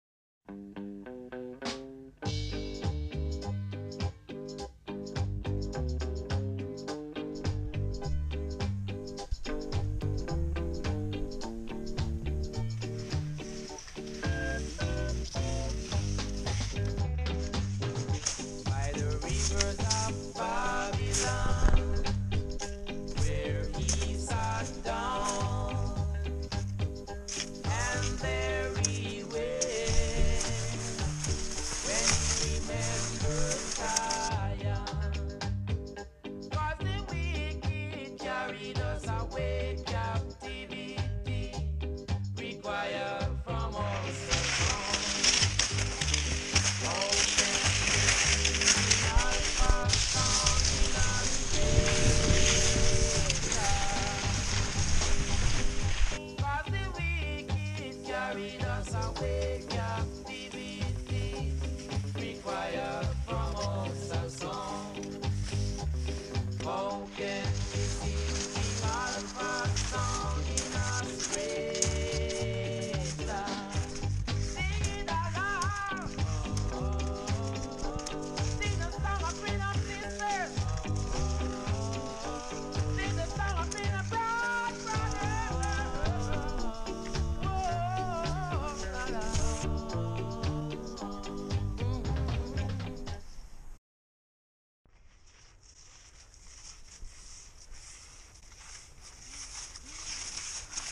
XC Ski January 2022
Skiing on the consolidated crust from earlier in the month.